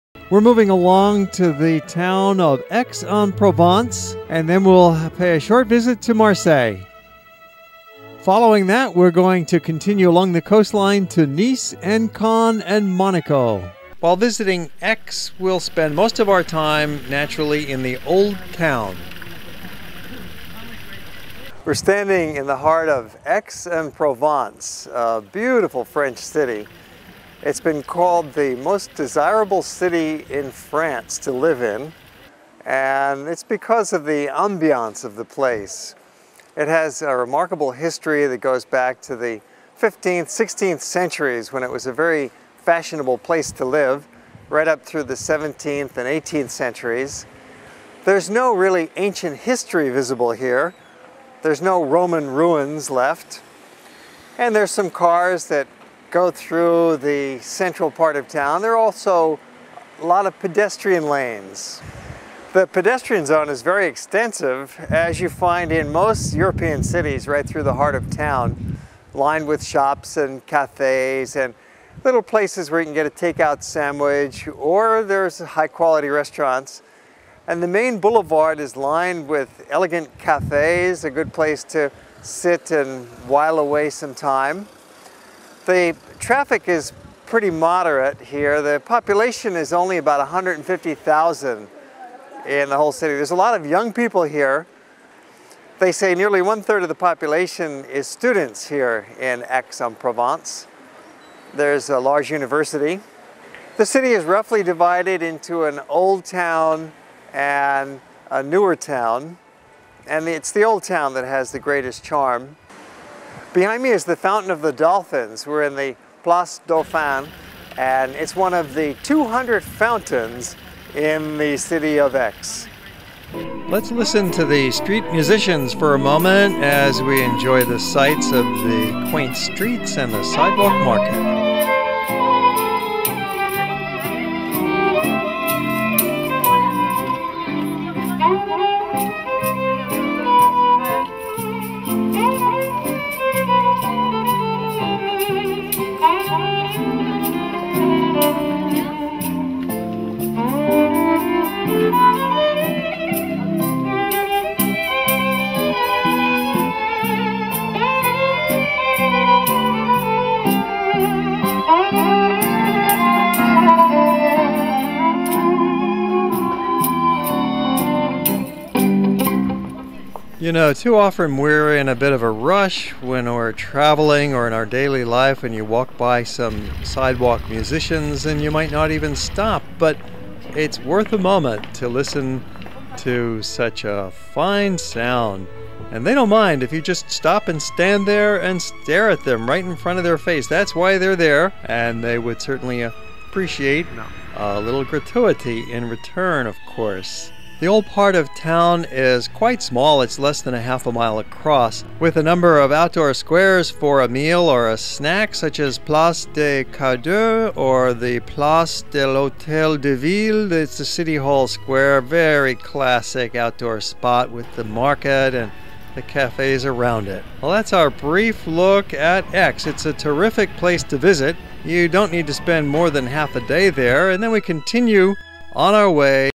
Aix en Provence 3 screens
Aix-en-Provence:  We visit the Old Town pedestrian zone of Aix: the outdoor produce market, Cours Mirabeau, La Rotonde, Place des Cardeurs, Rue Gaston de Saporta, Cathedrale St-Sauveur, Mazarin district. features three video screens running simultaneously